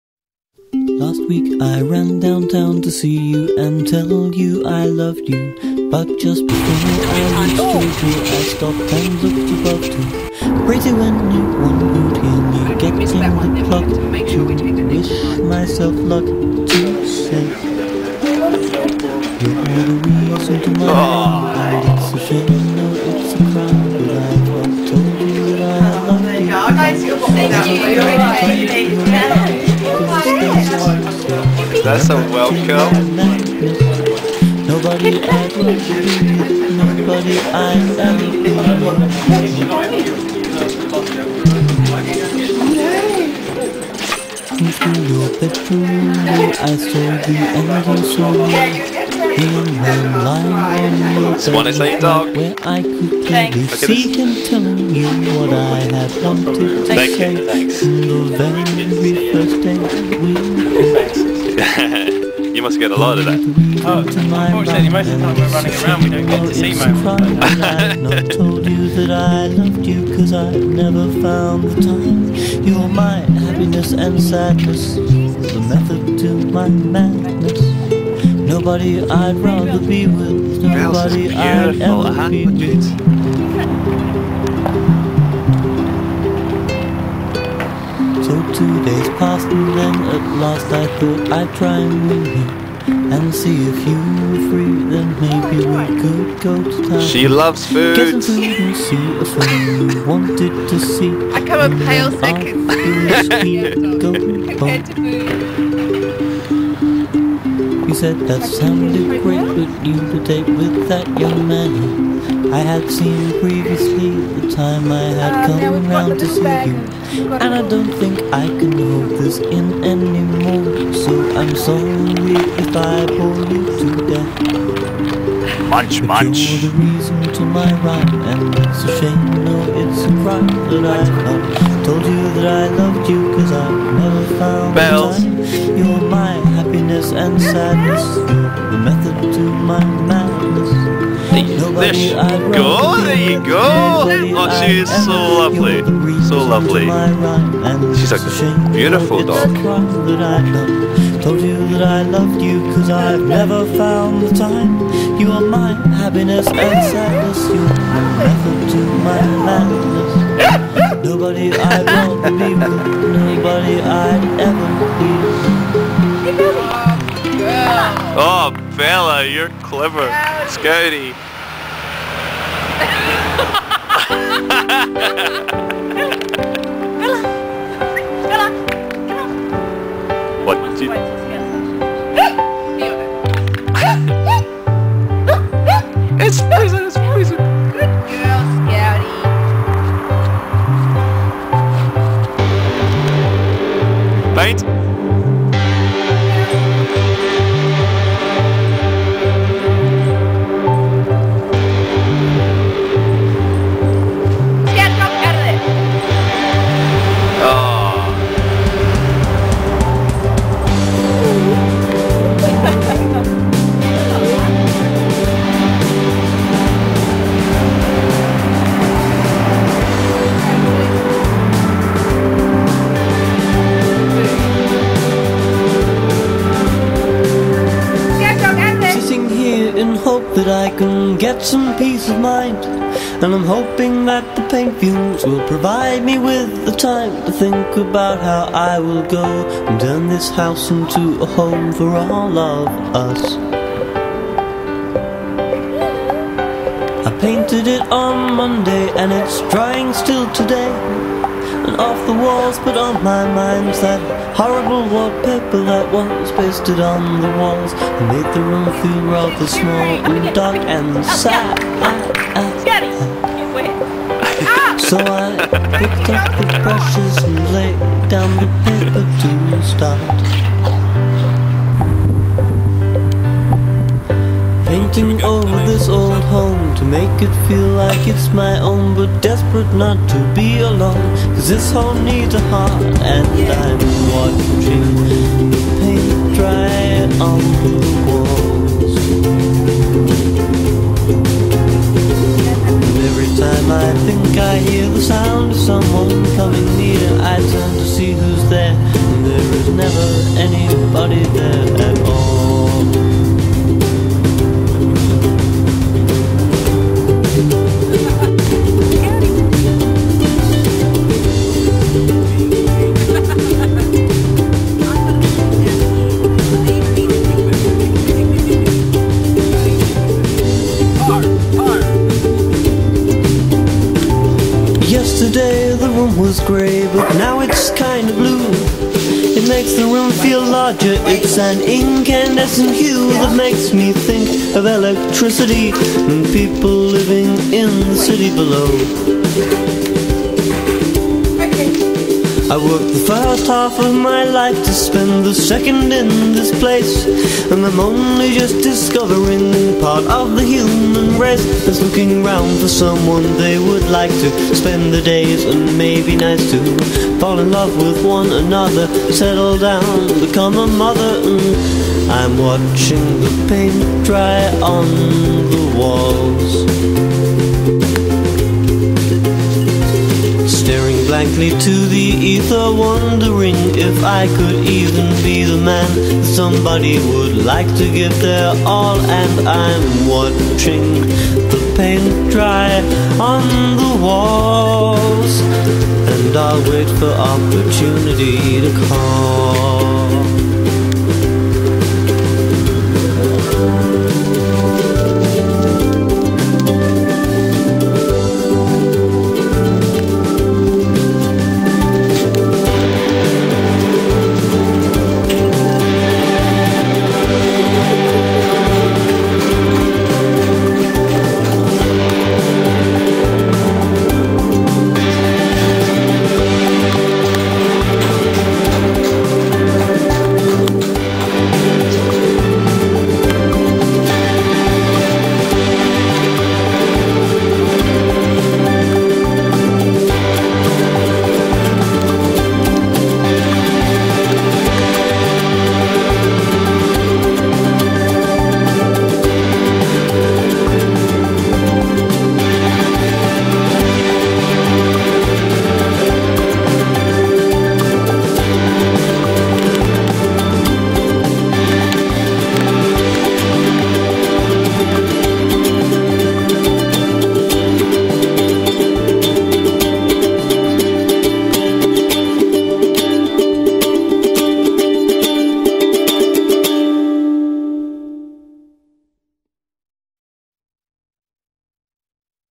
Bella and Scout arrive in Heathrow from Sydney
Here's our little film of Bella (Kelpie 15) and Scout (Vizsla 10) being reunited with their mothers having not seen each other for five months. The puppies made the mammoth journey from Sydney to London by plane. We caught a plane from Edinburgh to London - with our good friend Paul - and then the five of us drove home to Edinburgh.

Thanks to Tom Milsom for his lovely music, which can be found . Beautiful.